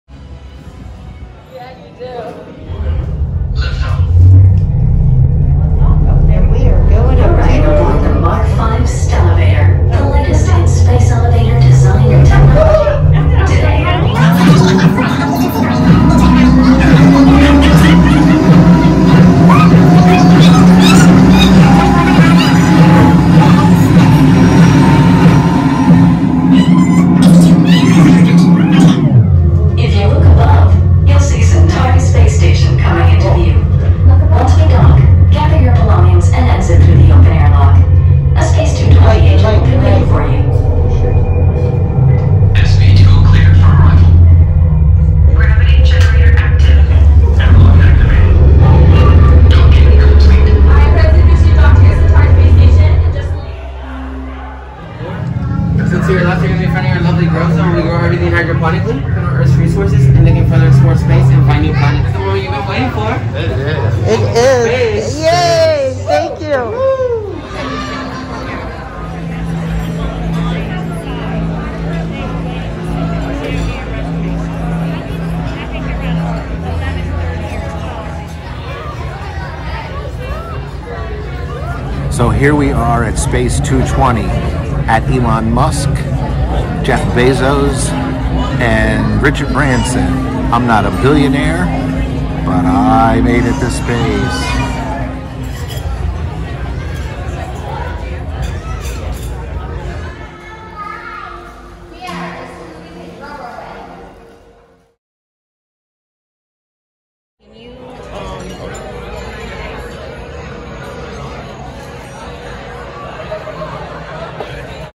WE WENT TO SPACE @ Elon Musk and we’re not billionaires…lol
This is a quick look at space 220 @ Epcot. A full dining review video will follow this one soon.
You too can go TO SPACE…